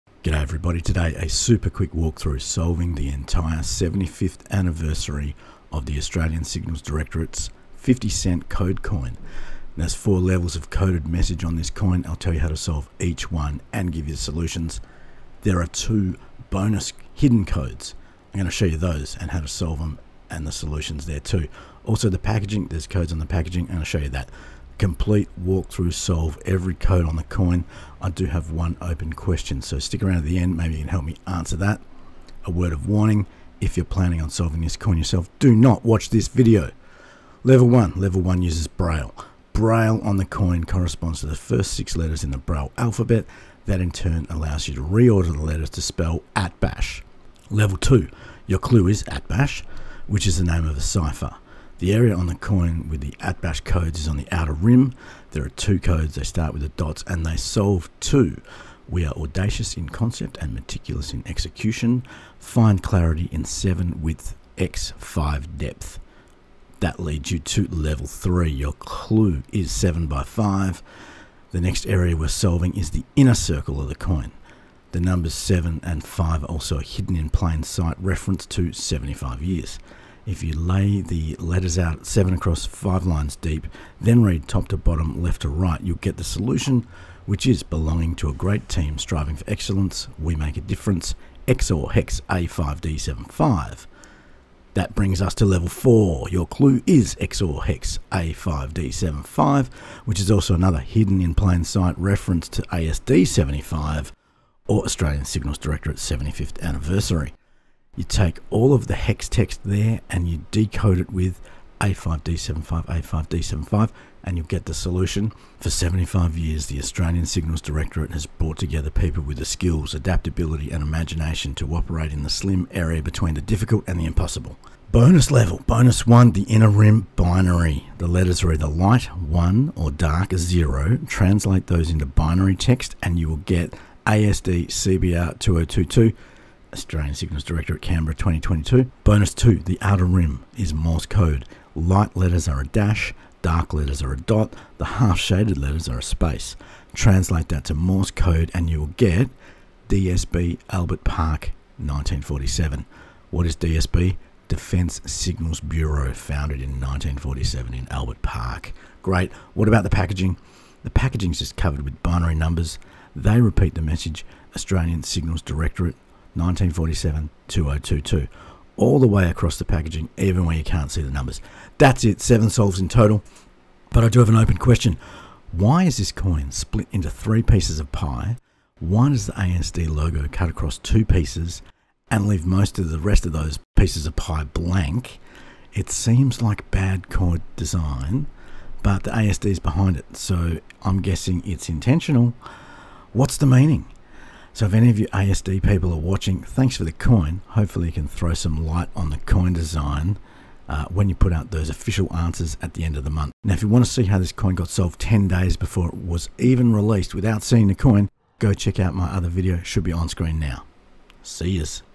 Australian Signals Directorate 50 cent coin full solve - 7 different codes solved ASD ASIO 75th
DO NOT WATCH IF YOU INTEND TO SOLVE THIS COIN YOURSELF!
Now that we've got our hands on the coin we can do a full solve.
All four levels of codes.
Both the two bonus codes,
and the packaging decoded.

IMO the ASD 75th code coin is better than the ASIO coin from 2019.
Plenty of crypto and cryptography and ciphers to keep the average person happy for hours! Certainly took me a few hours.